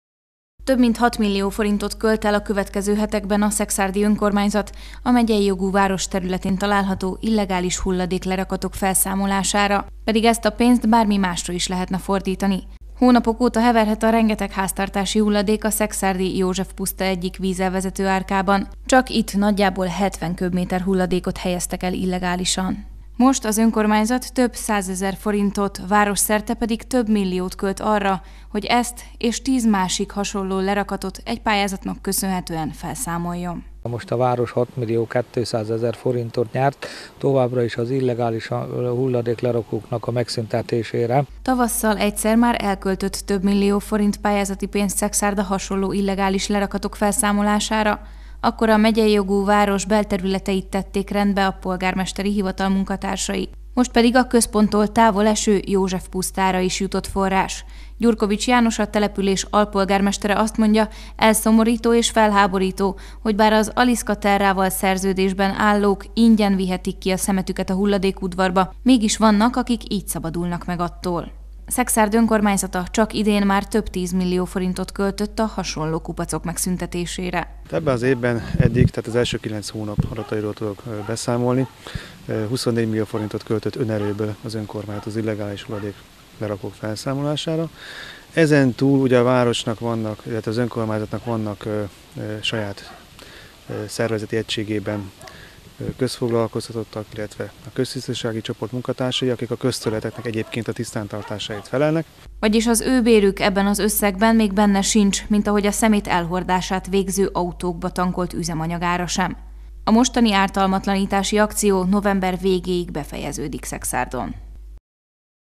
Illegális hulladékhalmok Szekszárdon
Több, mint 6 millió forintot költ el a következő hetekben a szekszárdi önkormányzat a megyei jogú város területén található illegális hulladék lerakatok felszámolására. Pedig ezt a pénzt bármi másra is lehetne fordítani. A forrásból 10 szeméthalmot tudnak felszámolni.